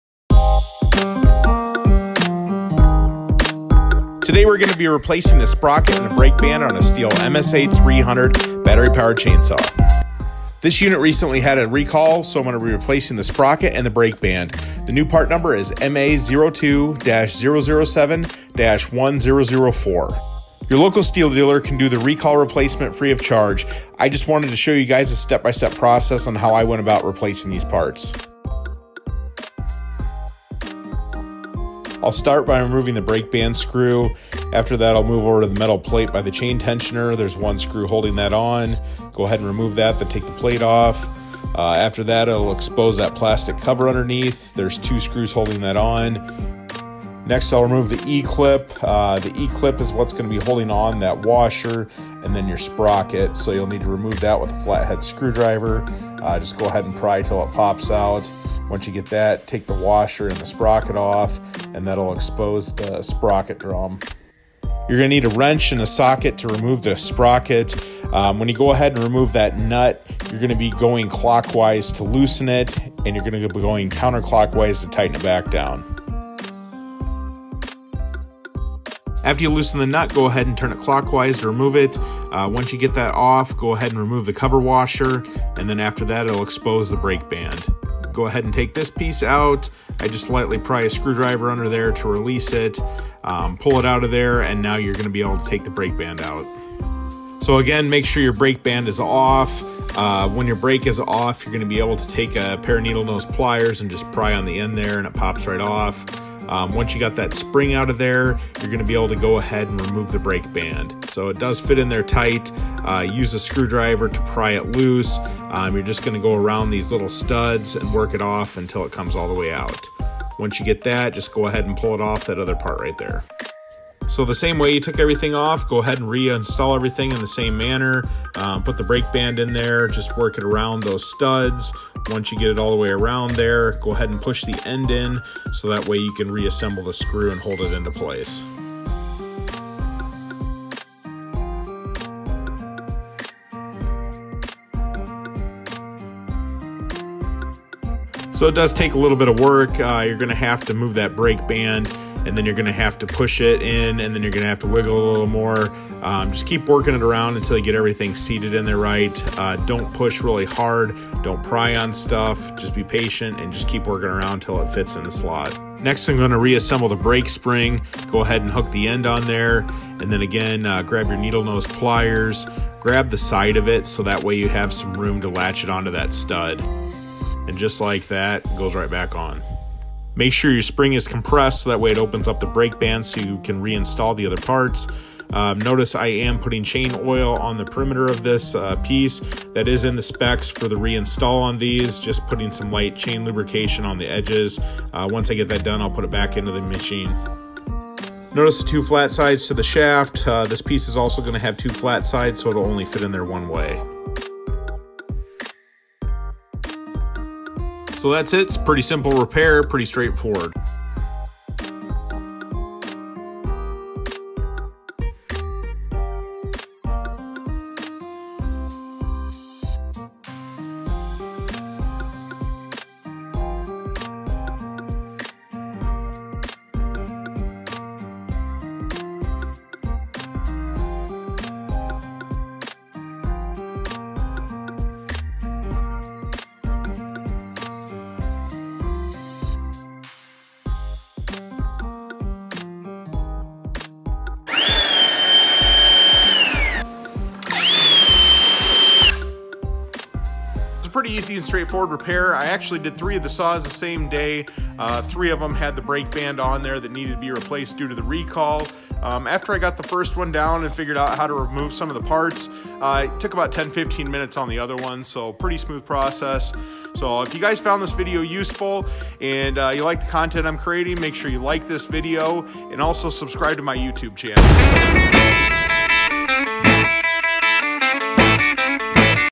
Stihl MSA 300 Battery Powered Chainsaw Break Band Recall. stihlmsa300 stihlbatterychai saw
An in-depth video of how to remove a recalled break band on a Stihl MSA 300 Battery Powered Chainsaw.  Step by step process and tips to help you remove the break band.